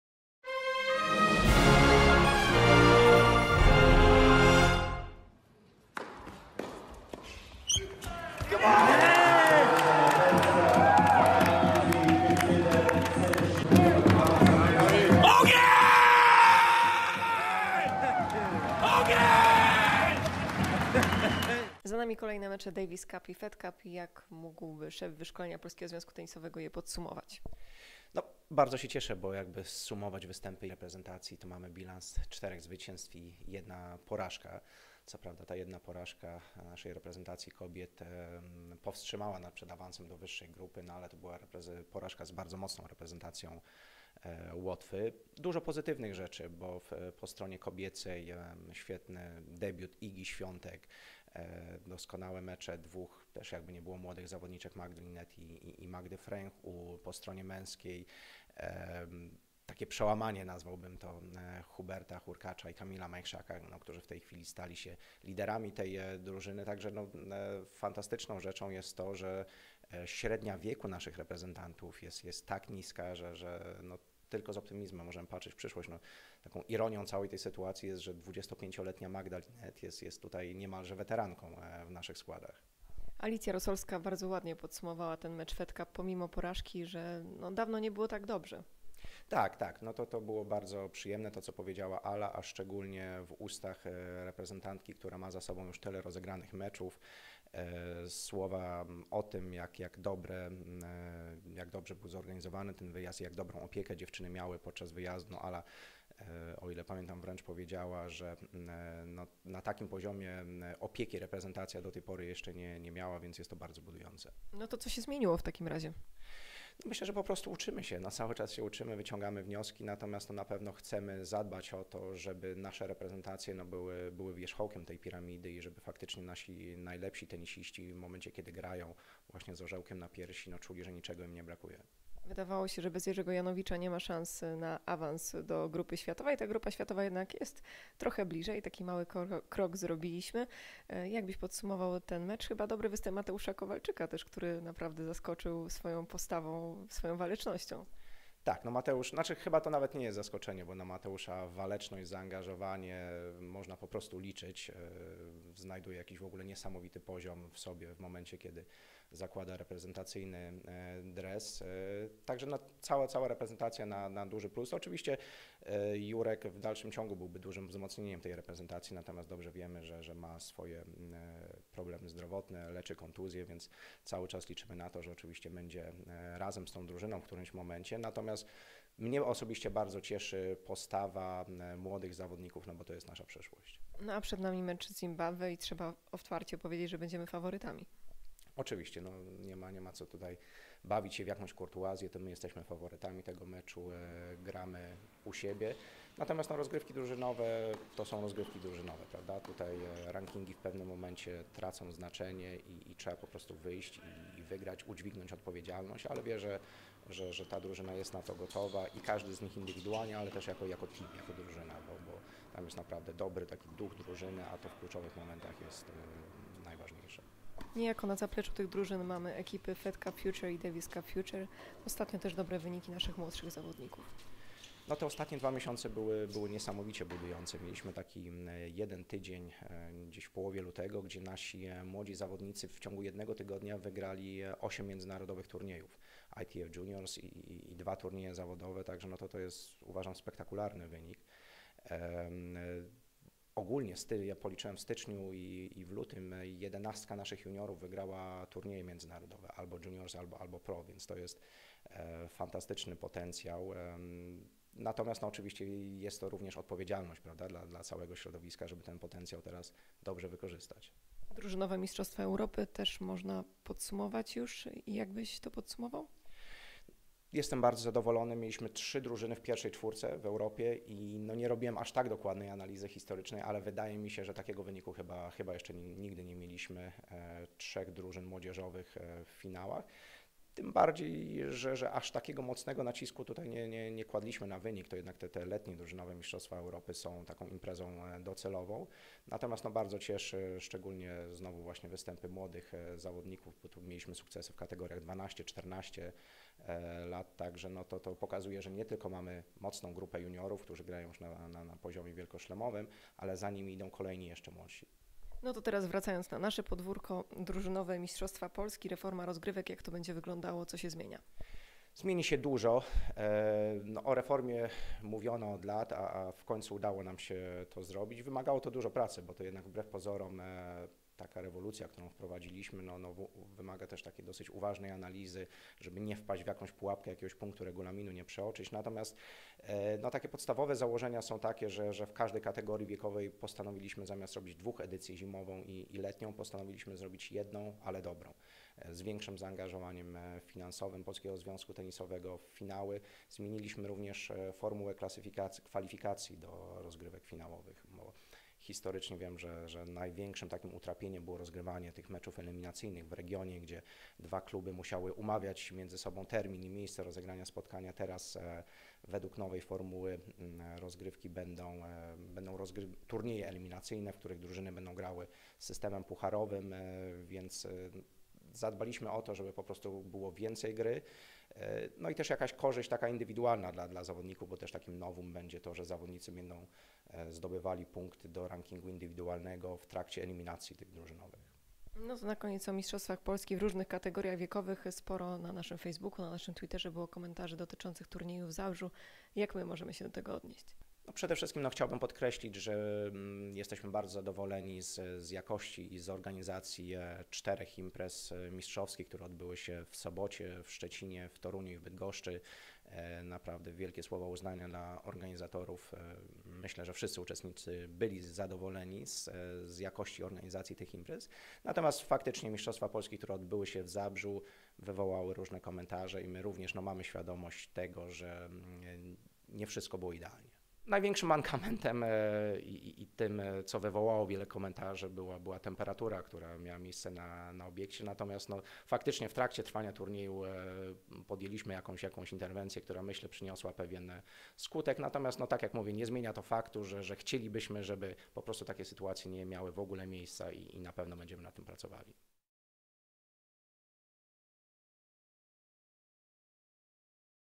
Zwycięstwo w Pucharze Davisa, sukcesy juniorów
Szef wyszkolenia Polskiego Związku Tenisowego, Rafał Chrzanowski, podsumował najważniejsze wydarzenia ostatnich tygodni. To był fantastyczny czas dla naszych juniorów. Z kolei seniorzy zrobili krok w kierunku Grupy Światowej Pucharu Davisa. Nie unikaliśmy też trudnych tematów - porażki w Fed Cup i problemów organizacyjnych, które dotknęły jedną z imprez mistrzowskich w Polsce. Przypominamy, że w PZT TENIS TV staramy się regularnie podsumowywać bieżące wydarzenia i odpowiadać na pytania. Zapraszamy do dyskusji!